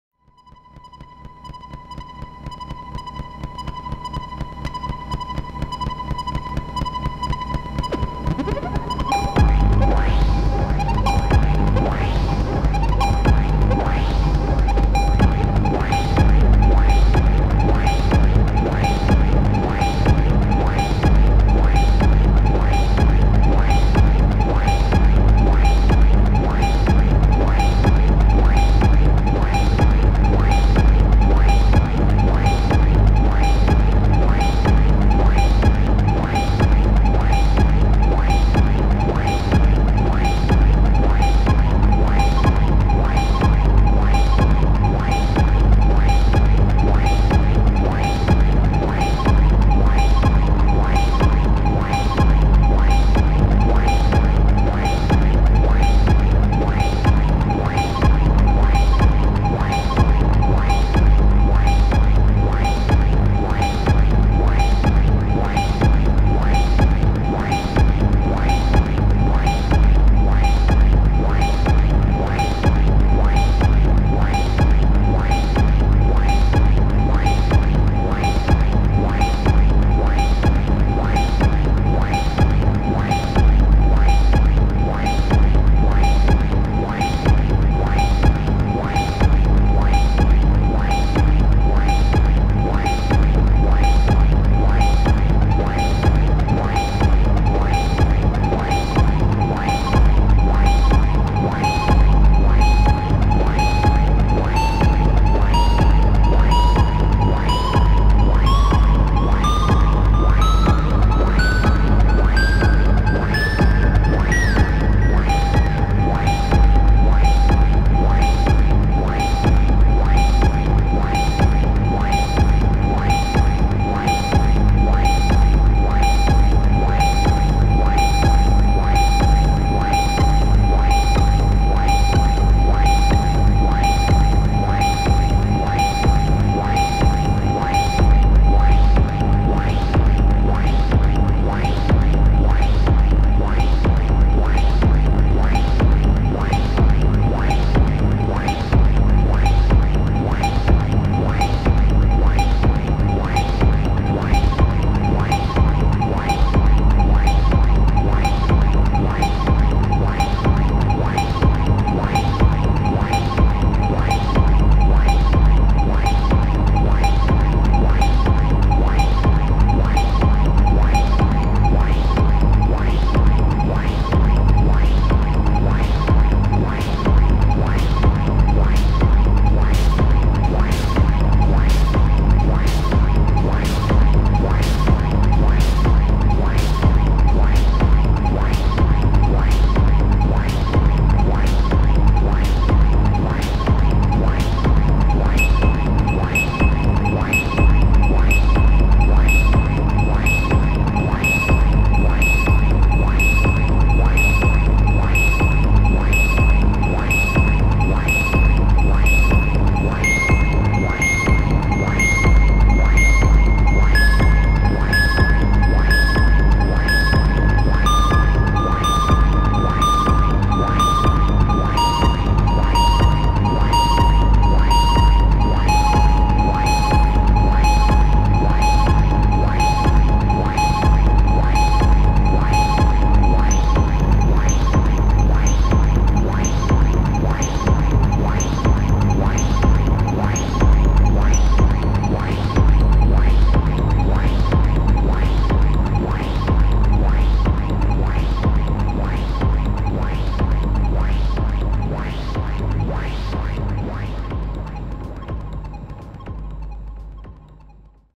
Batchas presents: Plumbutter - Patch 009
Play loud!

Plumbutter 1 + BugBrand DRM1

I divided yesterday's impro sesion in 3 parts. Patch 008/009/010.

Some higher parts are too loud compared to others. Like 1 x AVDog being much louder than the other - both controlled on the mixer by a unique pot. Same with Gongue.
So, I will at some point take all single outs to an external mixer and get a better control over each audio signal. Worth a try, even if this might be exactly this kind of "limitations" which make it sound so special.